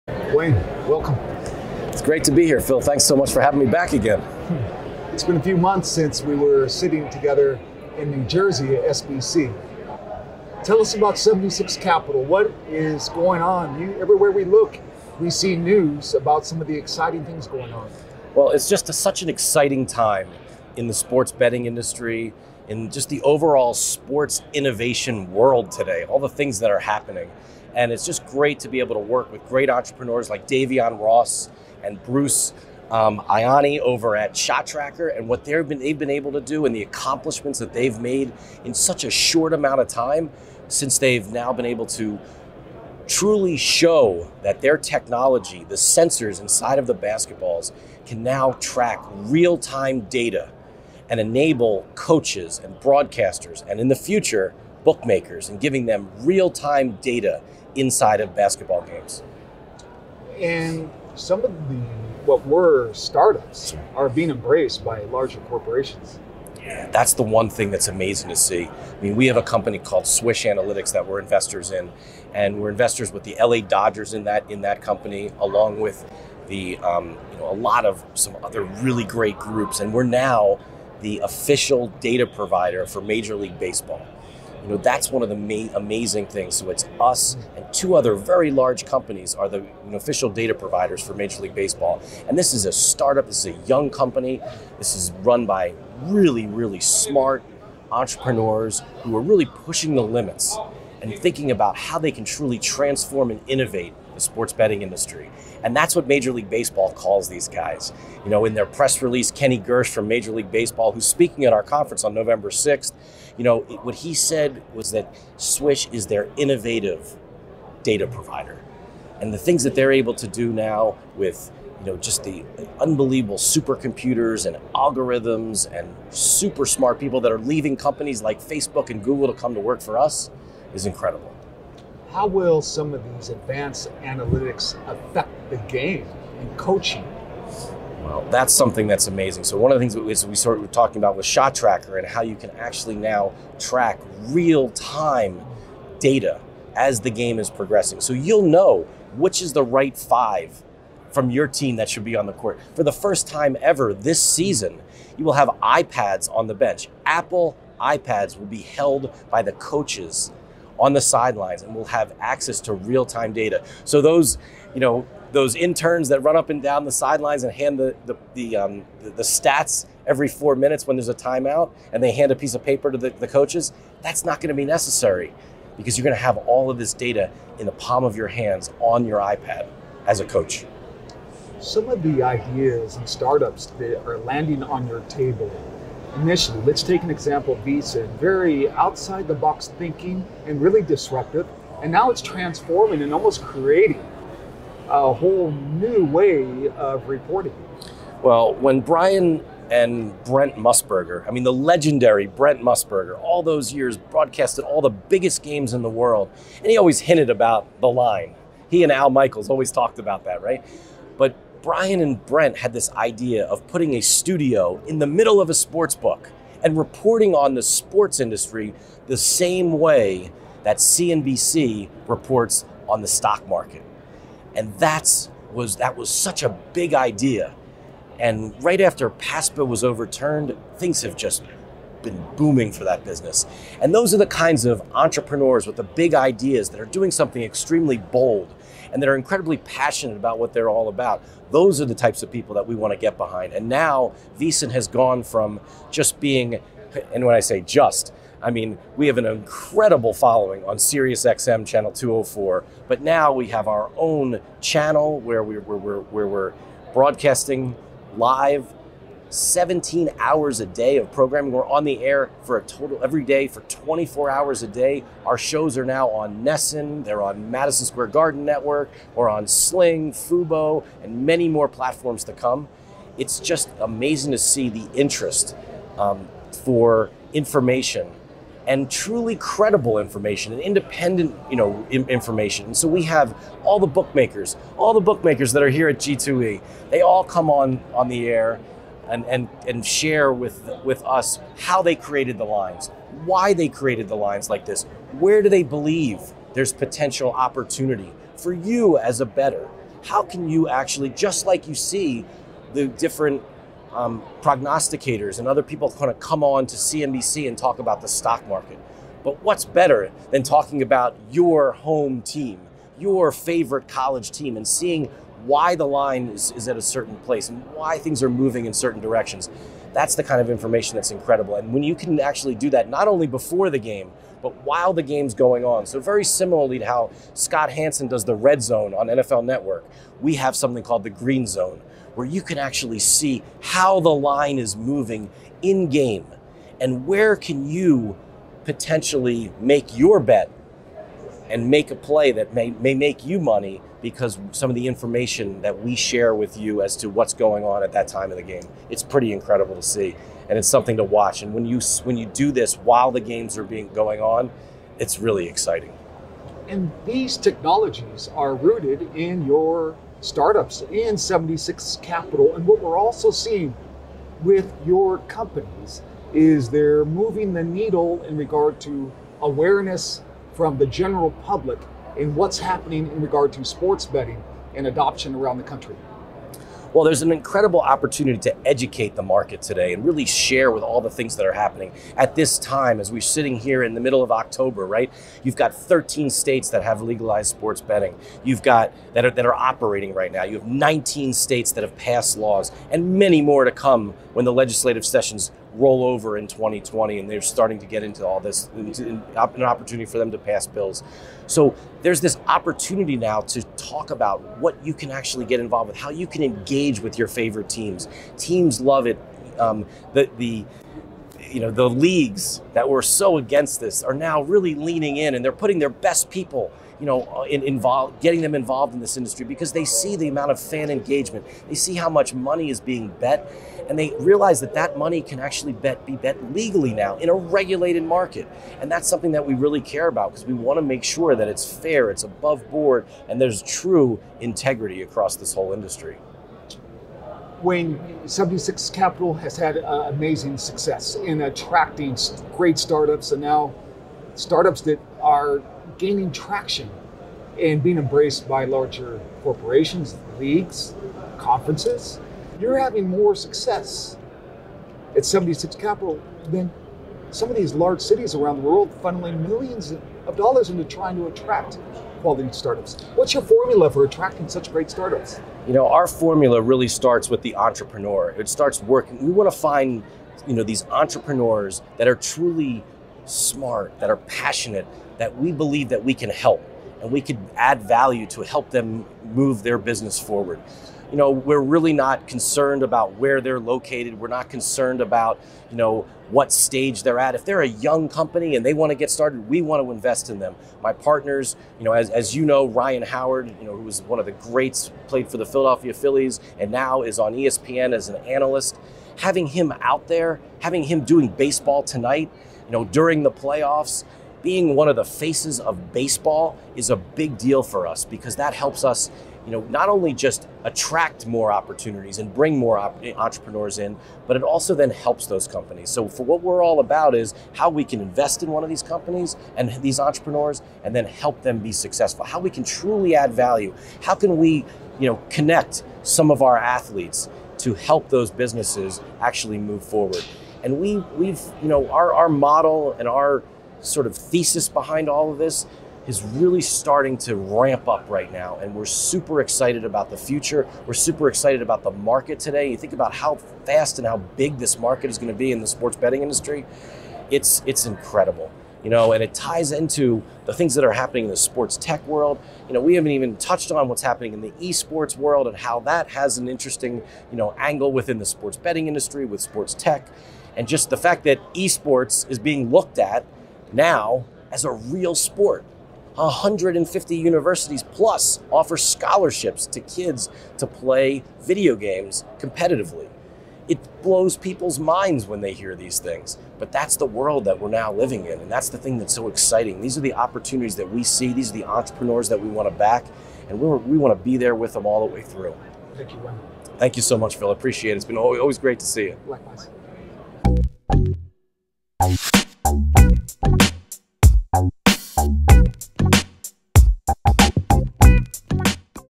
Wayne Kimmel Talks With Play+ during G2E Las Vegas 2019
Wayne Kimmel, Managing Partner at SeventySix Capital, talks with Play+ during G2E Las Vegas in 2019 about everything that is going on with SeventySix Capital and all of the portfolio companies it supports.